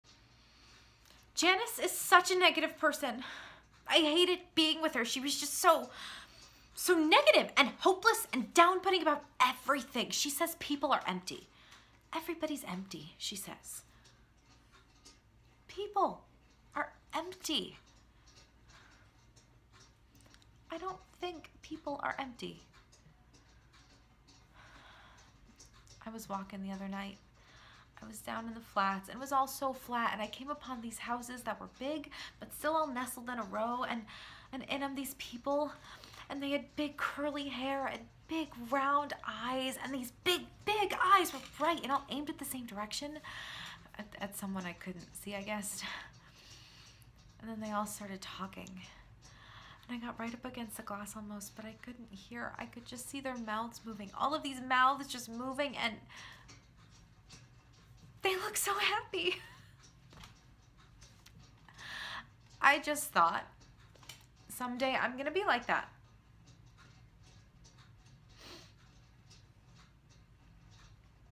Those the River Keeps
Lyndsay Crescenti performing Susie's monologue from Those the River Keeps by David Rabe.